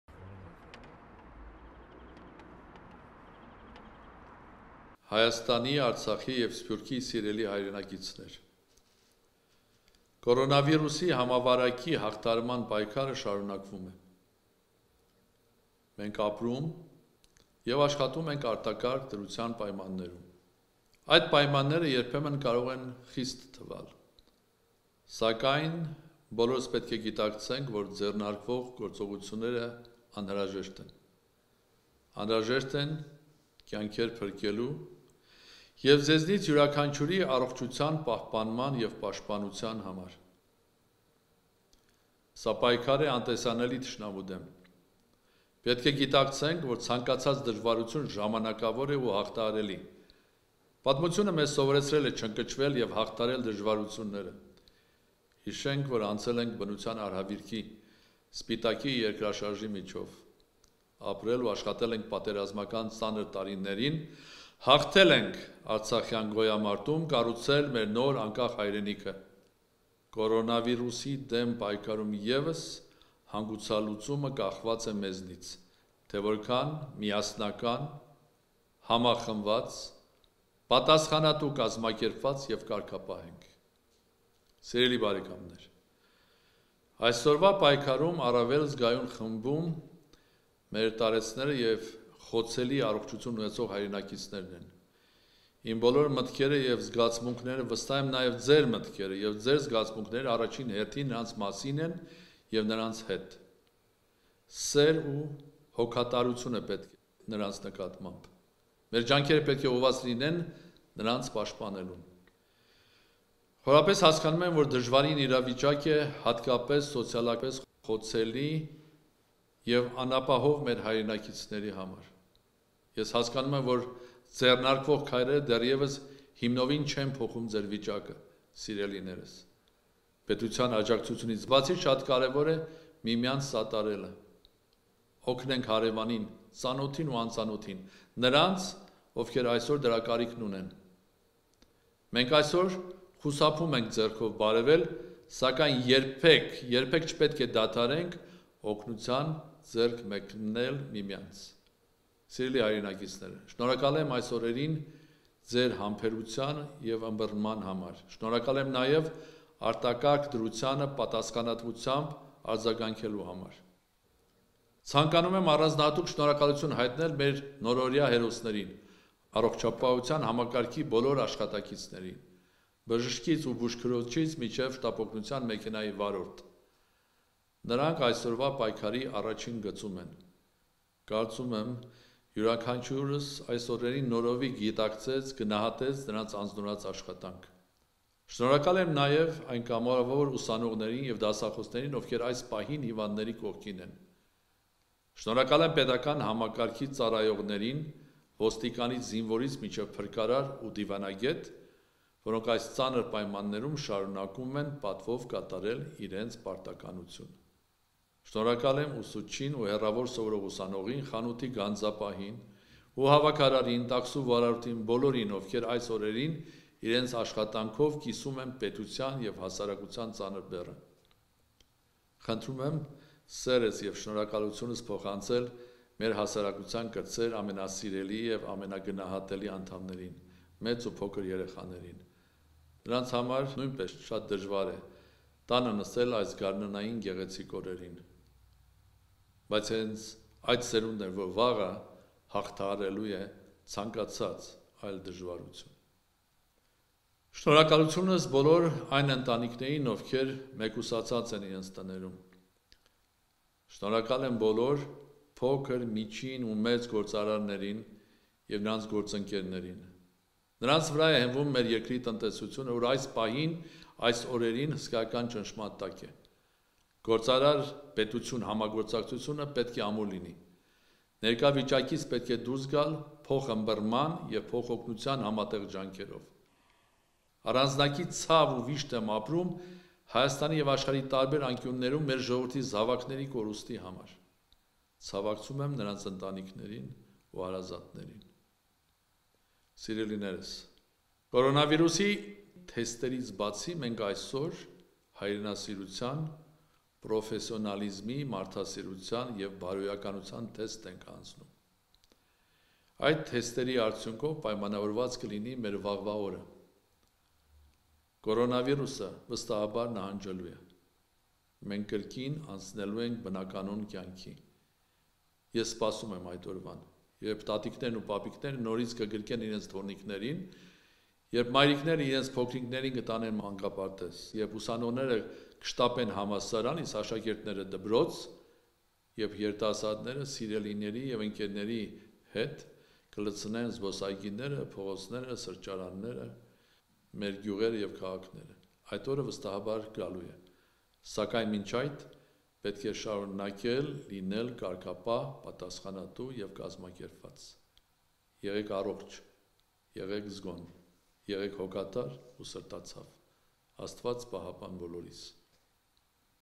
Հայաստանի Հանրապետության նախագահ Արմեն Սարգսյանի ուղերձը
The Office of the President of the Republic of Armenia

Հայաստանի, Արցախի և Սփյուռքի սիրելի՛ հայրենակիցներ,

Կորոնավիրուսի համավարակի հաղթահարման պայքարը շարունակվում է։  Մենք  ապրում և աշխատում ենք  արտակարգ  դրության պայմաններում:  Այդ պայմանները երբեմն կարող են  խիստ թվալ:

Սակայն բոլորս պետք է գիտակցենք, որ ձեռնարկվող գործողություններն  անհրաժեշտ են:

Անհրաժեշտ են կյանքեր փրկելու և ձեզնից յուրաքանչյուրի առողջության պահպանման եւ պաշտպանության համար:

Սա պայքար է անտեսանելի թշնամու դեմ:  Պետք է գիտակցենք, որ  ցանկացած դժվարություն ժամանակավոր է ու հաղթահարելի:

Պատմությունը մեզ սովորեցրել  է չընկճվել և հաղթահարել  դժվարությունները: Հիշենք, որ անցել ենք բնության արհավիրքի՝ Սպիտակի երկրաշարժի միջով: Ապրել ու աշխատել ենք պատերազմական ծանր տարիներին: Հաղթել ենք արցախյան գոյամարտում, կառուցել մեր նոր, անկախ հայրենիքը...

Կորոնավիրուսի դեմ պայքարում ևս հանգուցալուծումը կախված է մեզնից՝

թե որքան միասնական, համախմբված, պատասխանատու, կազմակերպված և կարգապահ ենք...

Սիրելի՛ բարեկամներ,

Այսօրվա պայքարում  առավել զգայուն խմբում մեր տարեցները և խոցելի առողջություն ունեցող քաղաքացիներն են:

Իմ բոլոր մտքերը և զգացմունքները, վստահ եմ, նաև ձեր մտքերը և զգացմունքները առաջին հերթին նրանց մասին և նրանց հետ են:

Սեր ու հոգատարություն է պետք նրանց նկատմամբ։

Մեր ջանքերը պետք է  ուղղված լինեն  նրանց պաշտպանելուն...

Խորապես հասկանում եմ, որ դժվարին իրավիճակ է հատկապես սոցիալապես խոցելի և անապահով մեր հայրենակիցների համար։

Ես հասկանում եմ, որ ձեռնարկվող քայլերը դեռևս հիմնովին չեն փոխում ձեր վիճակը:

Պետության աջակցությունից բացի շատ կարևոր է միմյանց սատարելը:

Օգնենք հարևանին, ծանոթին ու անծանոթին, նրանց, ովքեր այսօր դրա կարիքը ունեն:

Մենք այսօր խուսափում ենք ձեռքով բարևել:

Սակայն երբե՛ք, երբե՛ք չպետք է դադարենք օգնության ձեռք մեկնել միմյանց:

Սիրելի՛ հայրենակիցներ,

Շնորհակալ եմ  այս օրերին ձեր համբերության ու ըմբռնման համար:

Շնորհակալ եմ նաև  արտակարգ դրությանը պատասխանատվությամբ արձագանքելու համար:

Ցանկանում եմ առանձնահատուկ  շնորհակալություն  հայտնել մեր նորօրյա հերոսներին՝ առողջապահության համակարգի բոլոր աշխատակիցներին՝        բժշկից ու բուժքրոջից մինչև շտապօգնության մեքենայի վարորդ։

Նրանք այսօրվա պայքարի առաջին գծում են:

Կարծում եմ՝  յուրաքանչյուրս այս օրերին նորովի գնահատեց  նրանց  անձնուրաց աշխատանքը...

Շնորհակալ եմ այն կամավոր ուսանողներին և դասախոսներին, ովքեր այս պահին հիվանդների կողքին են:

Շնորհակալ եմ պետական համակարգի ծառայողներին՝ ոստիկանից, զինվորականից մինչև փրկարար ու դիվանագետ, որոնք այս ծանր պայմաններում շարունակում են պատվով կատարել իրենց պարտականությունները:

Շնորհակալ եմ ուսուցչին և հեռավար սովորող ուսանողին, խանութի գանձապահին ու հավաքարարին, տաքսու վարորդին, բոլորին, ովքեր այս օրերին իրենց աշխատանքով կիսում են պետության և հասարակության ծանր բեռը:

Խնդրում եմ սերս և շնորհակալությունս փոխանցել մեր հասարակության կրտսեր, ամենասիրելի և ամենագնահատելի անդամներին՝ մեծ ու փոքր երեխաներին:

Նրանց համար նույնպես շատ դժվար է տանը նստել գարնանային այս  օրերին:

Բայց հենց այդ սերունդն է վաղը հաղթահարելու ցանկացած այլ դժվարություն...

Շնորհակալությունս բոլոր այն ընտանիքներին, ովքեր մեկուսացած են իրենց տներում:

Շնորհակալ եմ բոլոր փոքր, միջին ու մեծ գործարարներին:

Նրանց վրա է հենվում երկրի տնտեսությունը, որը այս օրերին հսկայական ճնշման տակ է:

Գործարար-պետություն համագործակցությունը պետք է ամուր լինի։

Ներկա վիճակից պետք է դուրս գալ փոխըմռնման և փոխօգնության համատեղ ջանքերով։

Առանձնակի ցավ ու վիշտ եմ ապրում Հայաստանում և աշխարհի տարբեր անկյուններում մեր ժողովրդի զավակների կորստի համար:

Ցավակցում եմ նրանց ընտանիքներին ու հարազատներին։

Սիրելինե՛րս,

Կորոնավիրուսի թեստերից բացի մենք այսօր հայրենասիրության, պրոֆեսիոնալիզմի, մարդասիրության և բարոյականության թեստ ենք հանձնում:

Այդ թեստերի արդյունքներով պայմանավորված կլինի մեր վաղվա օրը:

 Կորոնավիրուսը, վստահաբար, նահանջելու է։

 Մենք կրկին անցնելու ենք բնականոն կյանքի:

Ես սպասում եմ այդ օրվան,

Երբ տատիկներն ու պապիկները  նորից կգրկեն իրենց թոռնիկներին,

Երբ մայրիկները իրենց փոքրիկներին կտանեն մանկապարտեզ,

Երբ ուսանողները կշտապեն համալսարան, իսկ աշակերտները՝ դպրոց,

Երբ երիտասարդները սիրելիների և ընկերների հետ կլցնեն զբոսայգիները, փողոցները, սրճարանները, մեր գյուղերն ու քաղաքները:

Այդ օրը, վստահաբար, գալու է:

Սակայն մինչ այդ պետք է շարունակել լինել կարգապահ, պատասխանատու և կազմակերպված:

Եղե՛ք առողջ, եղե՛ք զգոն: Եղե՛ք հոգատար ու սրտացավ:

Աստված պահապան բոլորիս...